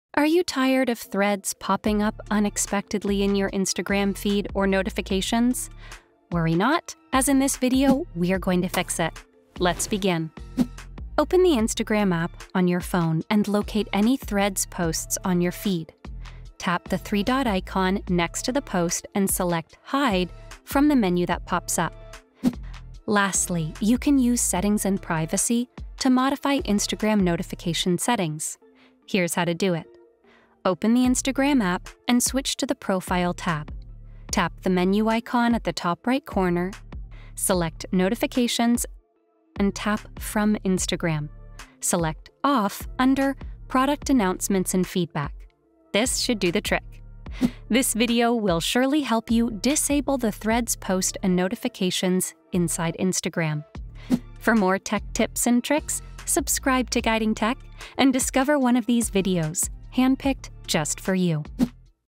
How to Stop Seeing Threads Posts and Notifications on Instagram | Guiding Tech
Are you tired of Threads popping up unexpectedly in your Instagram feed or notifications? You’re not alone. While reading Threads that interest you may be fun, its integration with Instagram can be overwhelming.

Fortunately, you can remove Threads from your Instagram feed and notifications tab if you wish to stop seeing them.

In this video, we will surely help you disable the threads post and notifications inside Instagram.